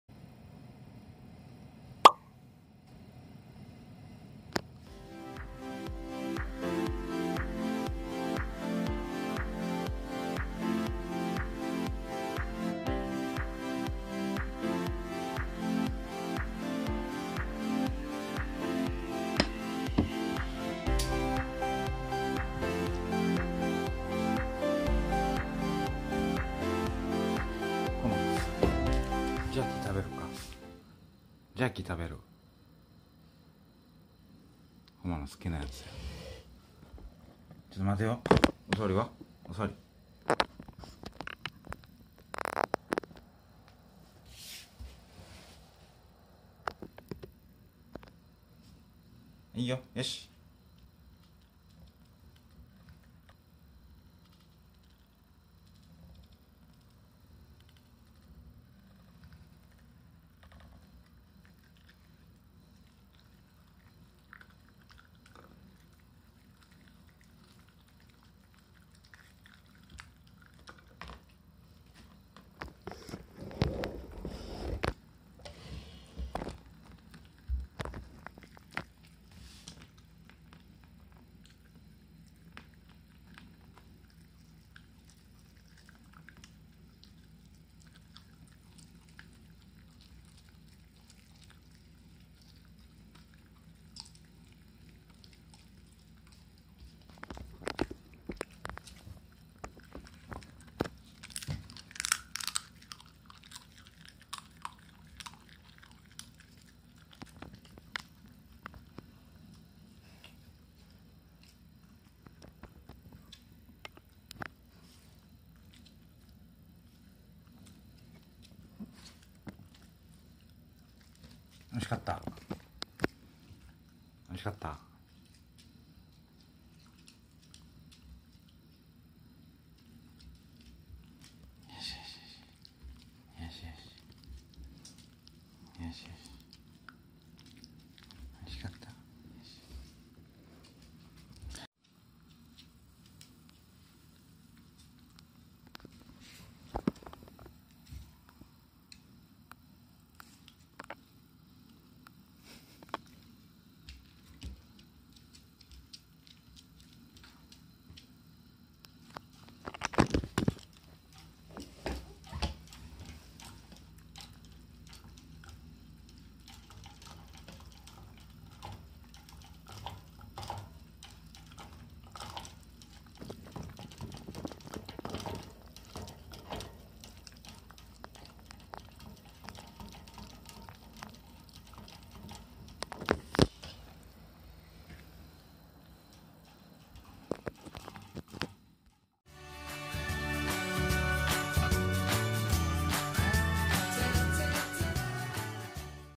手作りジャーキーVS市販のジャーキー柴犬こまの好みはどっちだ⁉【柴犬こま】shiba inu
柴犬こまのおやつにと鳥の胸肉で手作りジャーキーを作りました。めちゃくちゃ食いつきもよく気に入ってくれました。ふと思いました…　最近お気に入りの市販のジャーキーと手作りジャーキーを並べたらどっち選ぶのか…？
結果次第では、今後のジャーキー作りに対する私のモチベーションに関わってきます(笑)

Which do you like, homemade jerky or commercial jerky Shiba Inu?

関連動画
▶頑張ってルールを守ろうとする柴犬